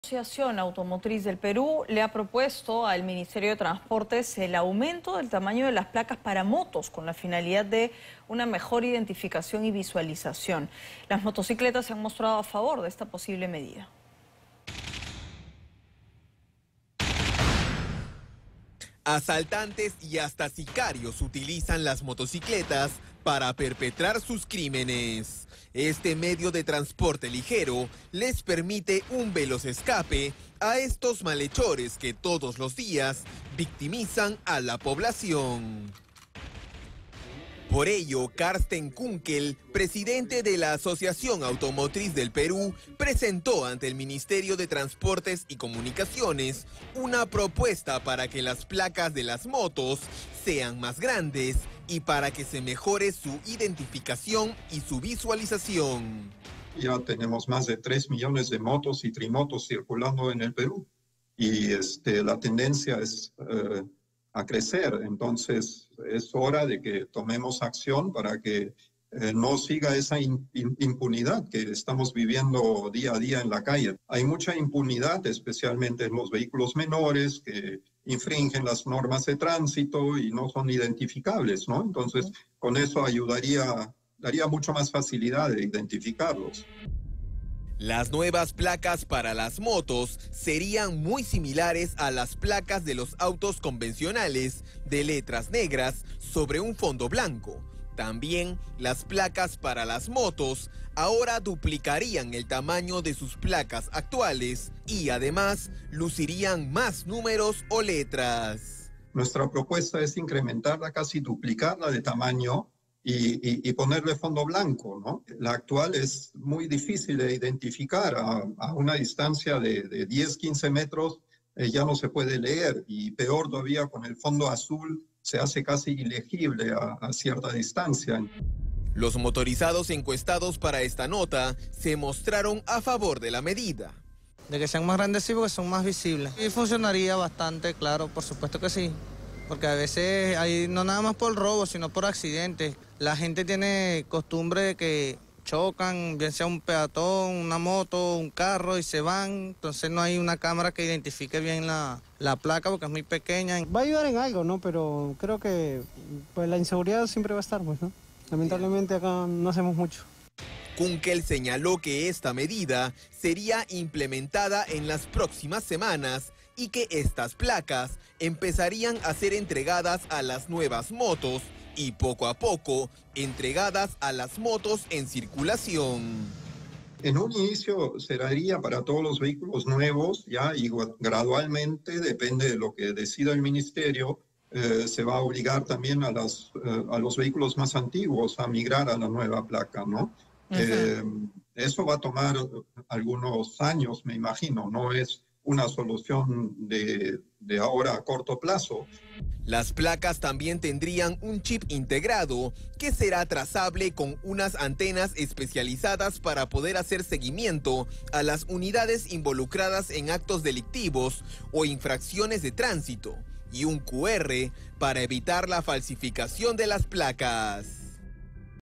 Aumento de tamaño de placas para motos: motorizados se muestran a favor de propuesta de AAP
La Asociación Automotriz del Perú (APP) ha presentado una propuesta al Ministerio de Transportes y Comunicaciones para aumentar el tamaño de las placas de las motocicletas, con el objetivo de mejorar su identificación y visibilidad. Esta medida busca combatir la impunidad en crímenes y violaciones de tráfico que involucran este tipo de vehículos, que son frecuentemente utilizados por delincuentes debido a su agilidad y rapidez.

Karsten Kunckel Saamer, presidente de la AAP, explicó que la propuesta incluye placas de mayor tamaño, con fondo blanco y letras negras, similar a las de los autos convencionales, facilitando su lectura a distancia. "Con más de 3 millones de motos y trimotos en Perú, y una tendencia al aumento, es crucial que tomemos medidas para evitar que esta situación de impunidad continúe", afirmó Kunckel.

Las nuevas placas serían casi el doble del tamaño actual y contarían con un chip integrado para seguimiento y un código QR para prevenir falsificaciones.

"La actual placa es muy difícil de identificar a una distancia de 10-15 metros, y el fondo azul la hace casi ilegible", señaló Kunckel Saamer. La implementación sería gradual, comenzando con los vehículos nuevos y extendiéndose eventualmente a los más antiguos, lo que implicaría un cambio a lo largo de varios años.

SE MUESTRAN A FAVOR

Por su parte, los motorizados consultados mostraron su apoyo a la iniciativa y reconocieron que placas más grandes podrían ayudar no solo en la identificación en casos de delito, sino también en accidentes de tráfico.

Esta propuesta aún espera la aprobación y la implementación por parte del Ministerio, y está abierta a comentarios públicos antes de su oficialización y puesta en marcha.